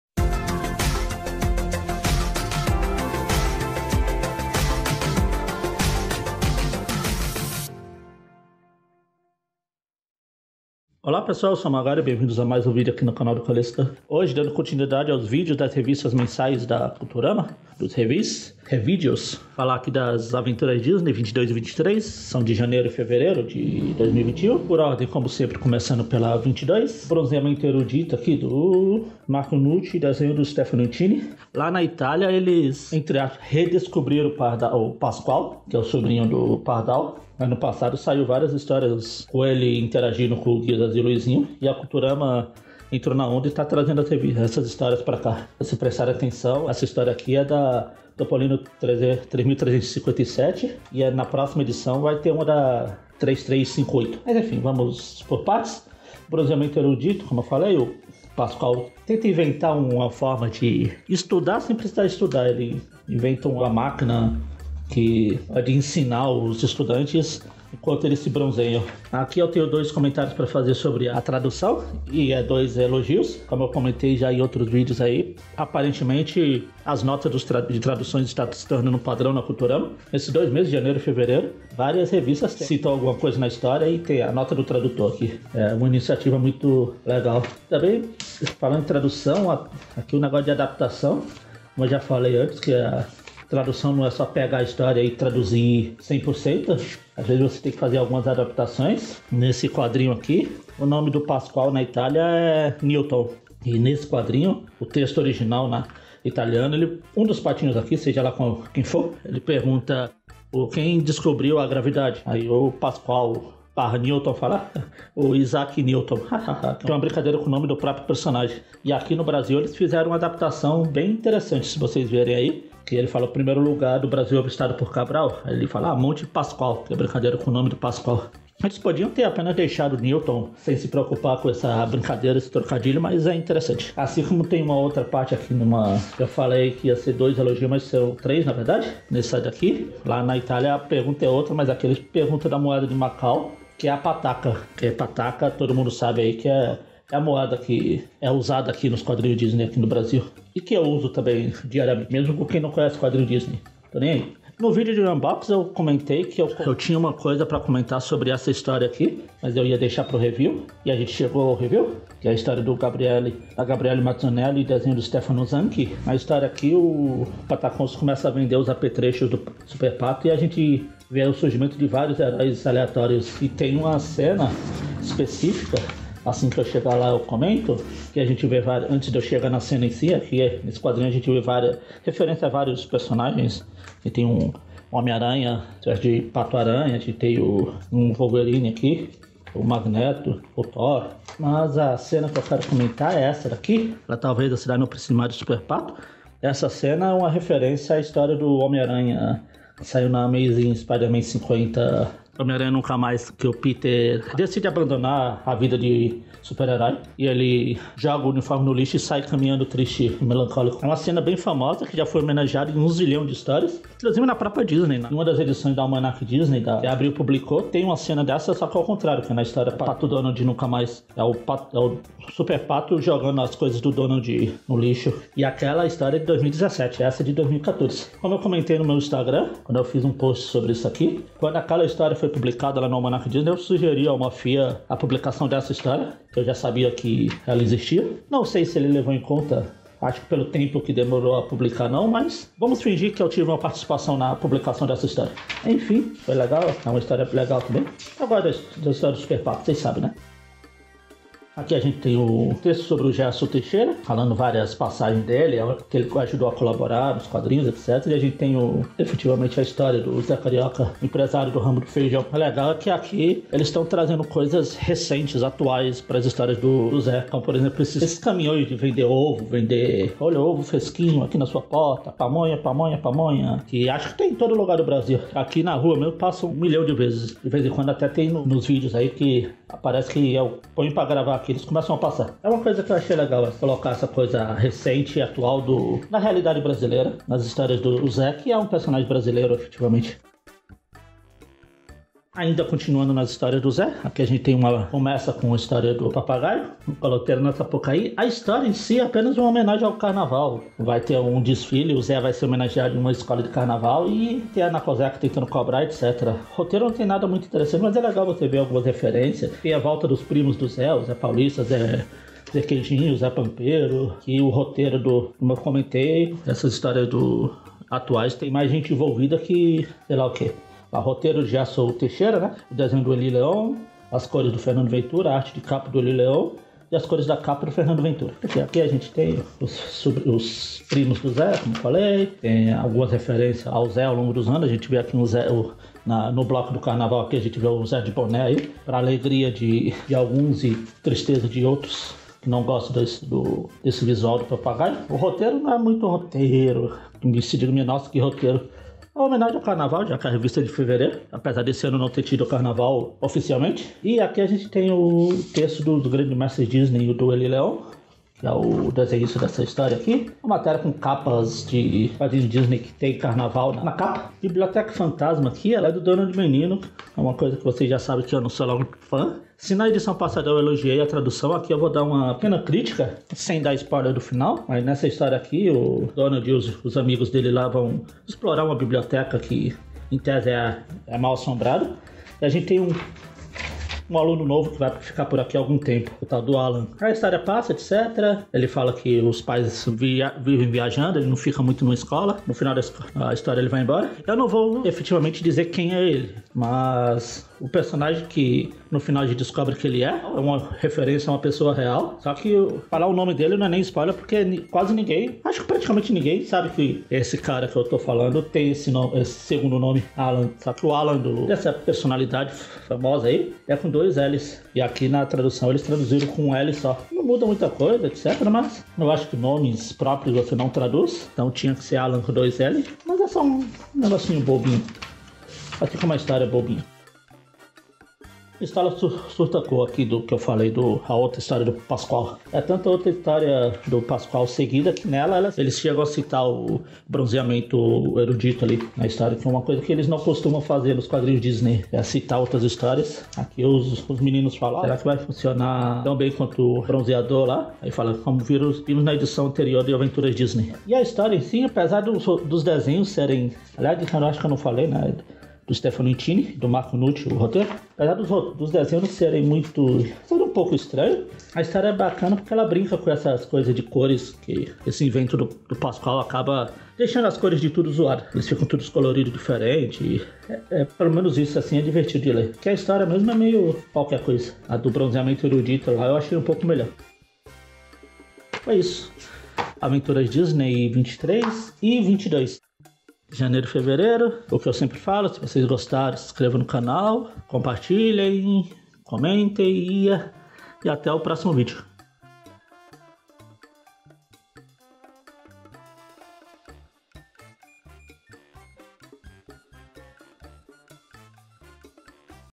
Review: Aventuras Disney edições 22 e 23.
Vídeo com comentários das edições 22 e 23 da revista Aventuras Disney, lançadas pela Editora Culturama em Janeiro e Fevereiro de 2021.